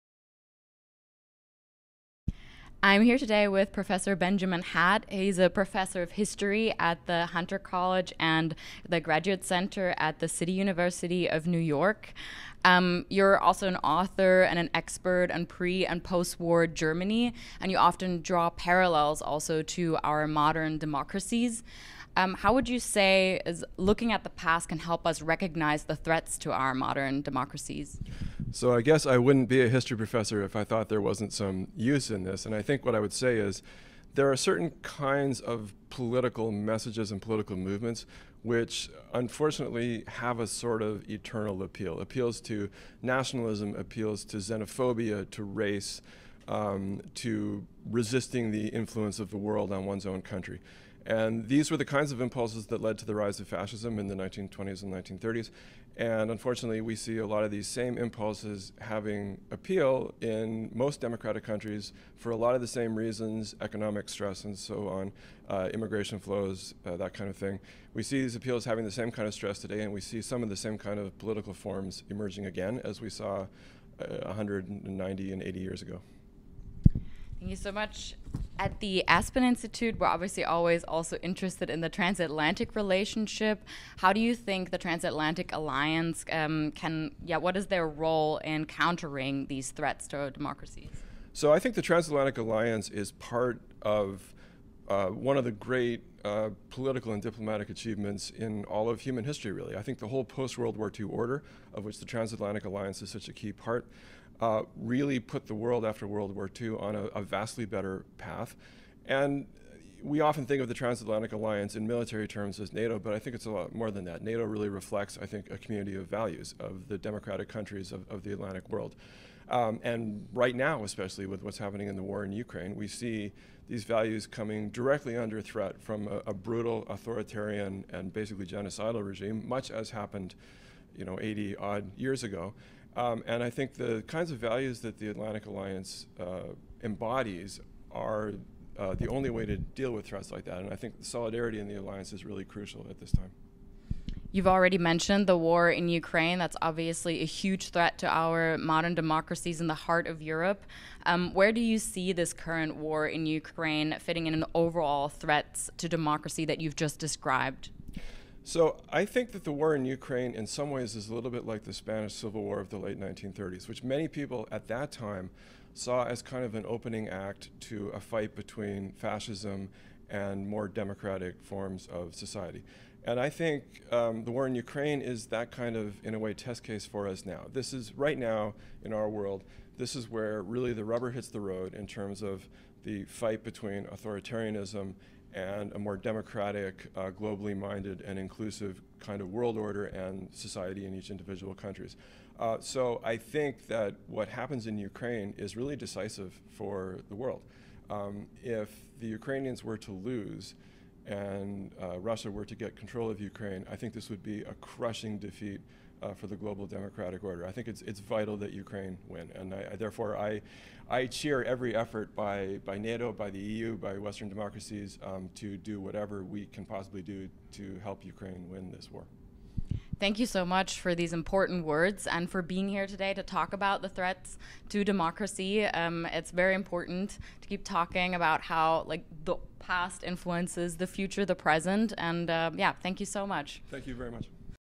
Global Threats Series Interview 9 - Prof. Benjamin Hett on the threats to democracy
On June 5th, 2023, we interviewed Prof. Benjamin Hett, Professor of History at the Hunter College and the Graduate Center of the City University of New York. Prof. Hett explained how looking at the past can give us insights into modern dynamics which threaten our democracies.